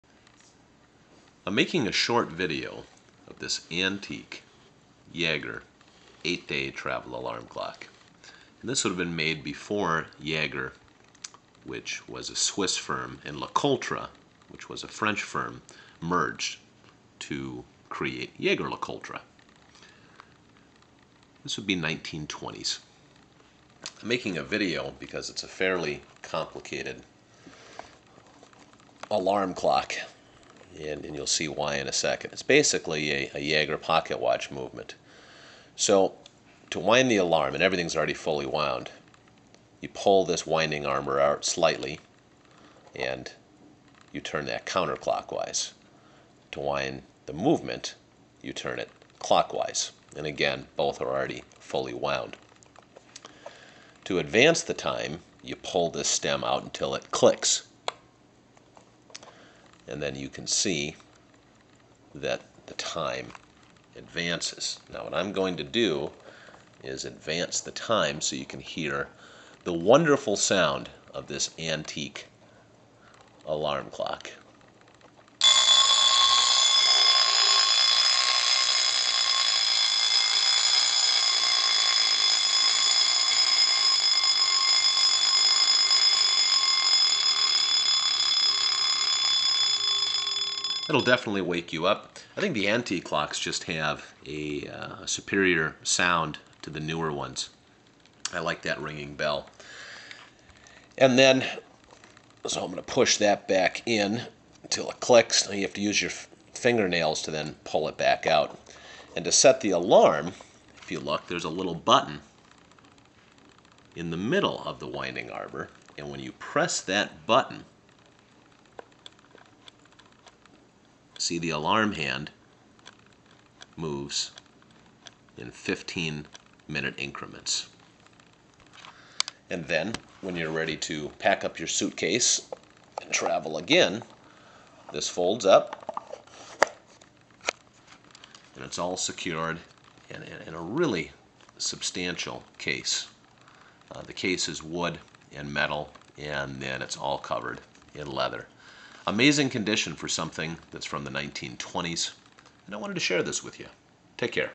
Antique Jaeger travel alarm clock
This was made by the Swiss firm (Jaeger) before they officially merged with the French firm (LeCoultre) in 1937.  It is basically a Jaeger pocket watch mounted inside a wood, metal and leather travel case.